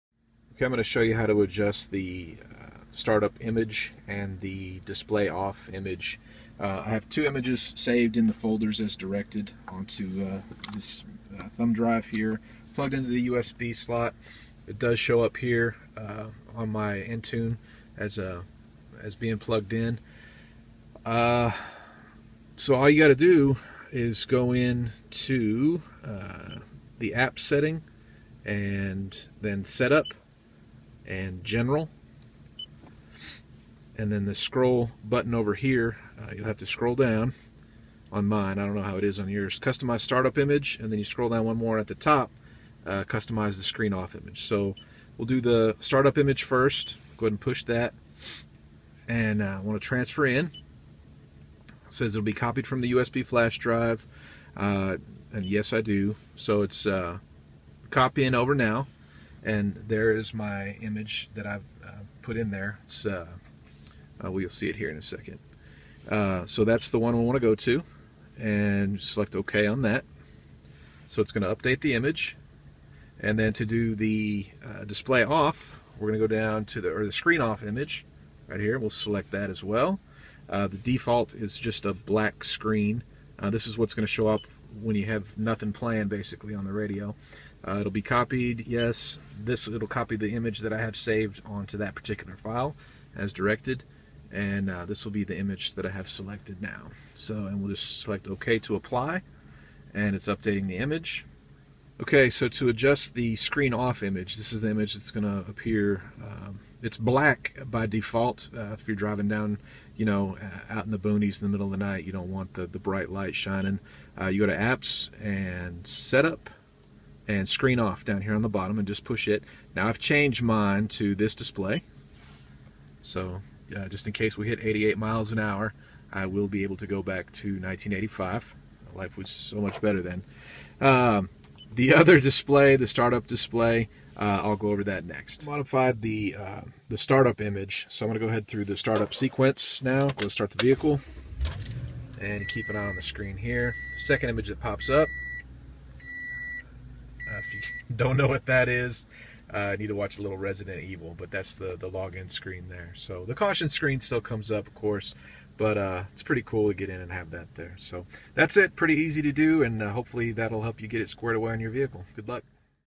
How To Customize 2014 Tundra Entune Display Images
Short video walk through of how to add both a custom Startup Image and custom Display Off Image on a 2014 Toyota Tundra with Entune entertainment system.

I personally set the maximum width to 885 pixels for both the images I use in this video, which seemed to work well.  Remember that .jpg or .jpeg files must be used, and they must be placed in the StartupImage and DisplayOffImage folders (respectively) on the root directory of your thumb drive.  Spelling of the folder names must be exact or they will not be recognized by Entune.

If you have questions, there are instructions on how to do this in your manual.  Check it out.

Thanks to 14TundraTRD and everyone else on the TundraTalk.net forum for guidance.

I've never tried to post any kind of how-to video before, so please be mindful of that in your comments.  Enjoy!